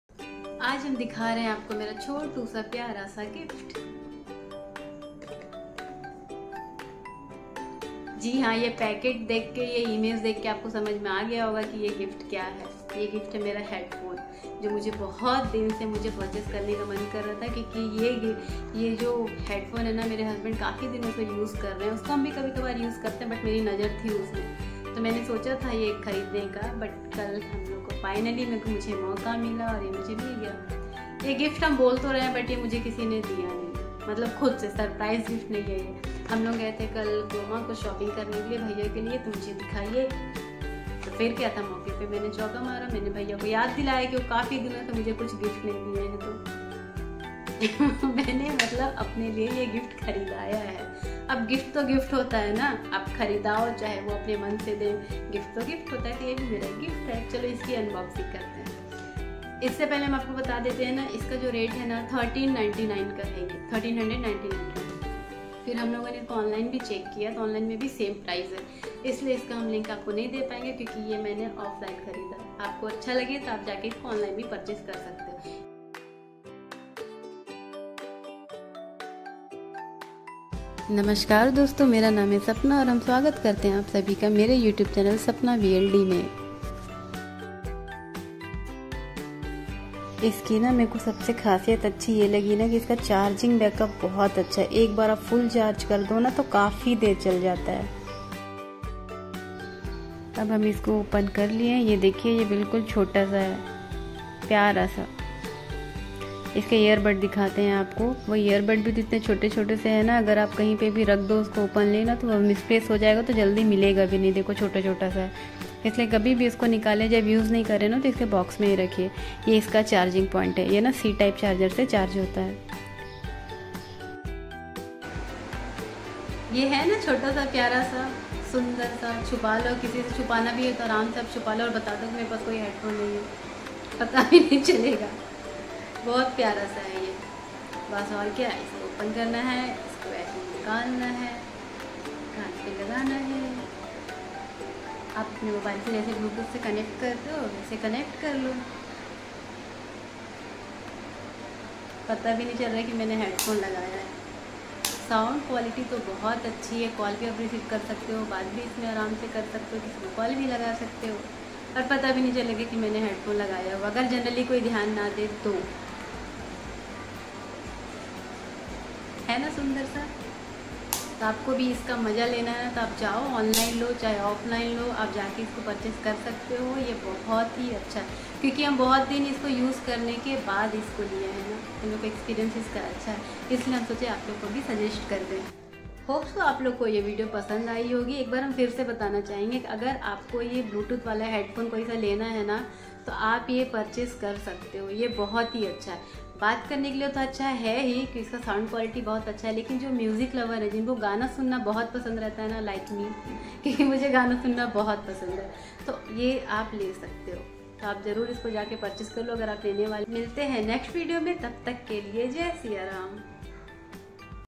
|UNBOXING:-Realme TechLife Buds T100|Battery Life 24Hr|Real Hd Sound👍Mujhe Bahot Pasand Aya🥰🥰 Nice 👍
Hello Everyone Jai shree Ram 🙏🙏🙏

|Realme TechLife Buds T100|Battery Life 24Hr|Real Hd Sound|Nice Product 👍Mujhe Bahot Pasand Aya🥰🥰

Bahot Hi Achha Product Hai Ye Gift Hum Khud Se Gift Karwayen Hain Bhaiya Se🤩🤩
Jaruri Hai .

Ye Product Online Ofline Dono Se Aap Purchase Kar Sakte ho Same Rate Me.

Realme TechLife Buds T100
:HD SOUND
:BLACK COLOUR FULL MATTE
: BATTERY LIFE 24HR
:FAST CHARGING
: INTELLIGENT TUCH CONTROL
:PRICE 1399/-

Baki Feature To Aap Use Kark Samjh Jaoge Agr Apko Purchase Karna To Ye Product Kar Sakte Ho😊😊